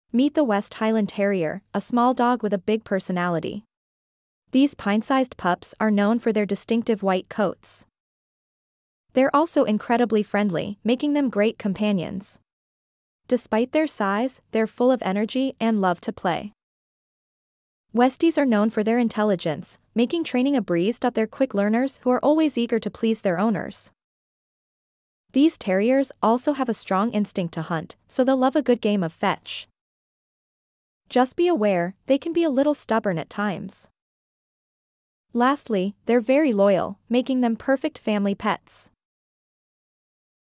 The Naked Pooch - West Highland Terrier
The West Highland White Terrier, often called the "Westie," is a small and charming dog breed originating from Scotland. Known for its distinctive white coat, this breed is compact and sturdy, typically weighing around 15-20 pounds and standing about 10-11 inches tall. With a playful and lively personality, Westies make delightful companions and are well-suited for both city and country living. Their double coat consists of a dense undercoat and a harsh outer layer, requiring regular grooming to maintain their appearance.

Despite their small size, Westies are confident and spirited, possessing a strong sense of curiosity. They are generally friendly towards people, including children, and can coexist well with other dogs when properly socialized. However, their terrier roots can make them assertive and prone to chasing small animals. This breed is intelligent and can be trained, although they might exhibit a touch of stubbornness at times. With their endearing looks and spirited character, West Highland White Terriers make for lively and affectionate companions in various households.